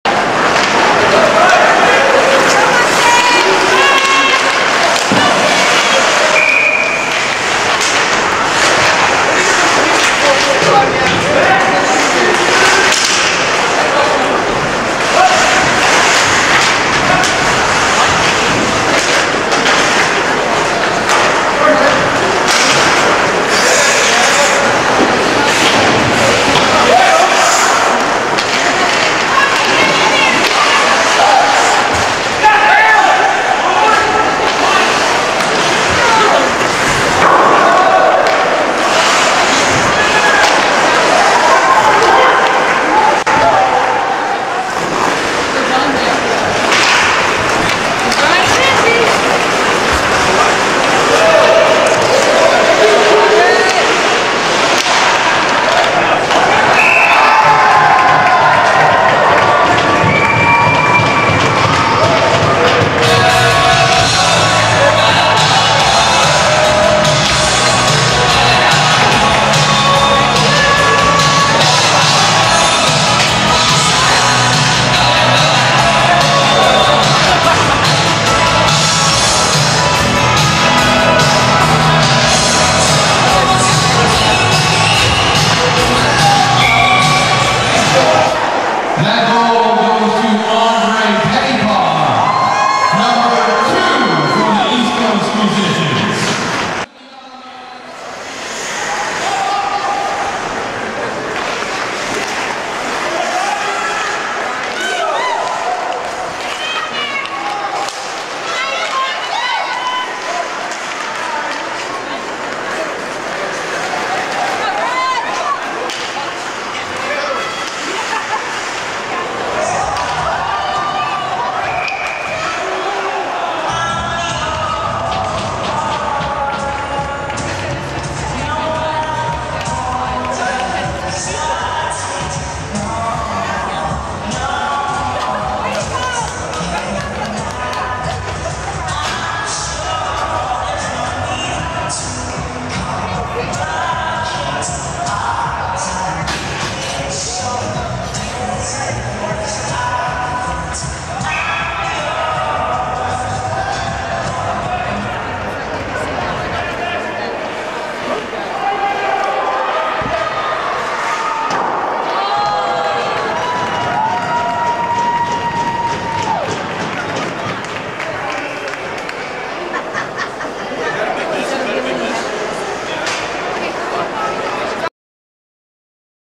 East Coast Musicians Hockey Game - Highlights 03 - April 26 2012.mpg
East Coast Musicians played a fun hockey game against the East Antigonish East Academy Teachers and Students at the Antigonish Arena on April 26, 2012.  All proceeds to the Student Council.  Pettipas scores and Greg Morrow dives and gets a penalty shot.